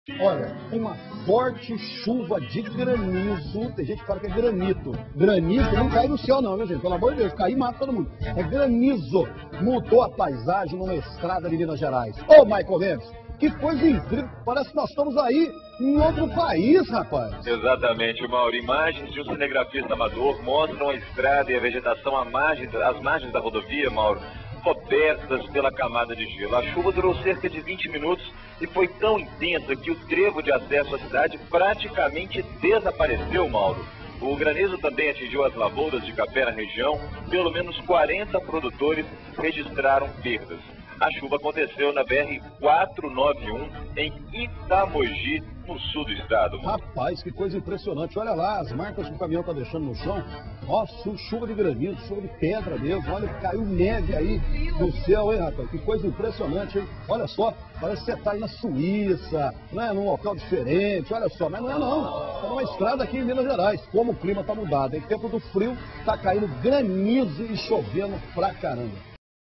Chuva de granizo destrói lavouras e cafezais no sul de Minas
Uma chuva de granizo danificou plantações em várias cidades do sul de Minas. A lavoura de café do ano deverá ficar prejudicada.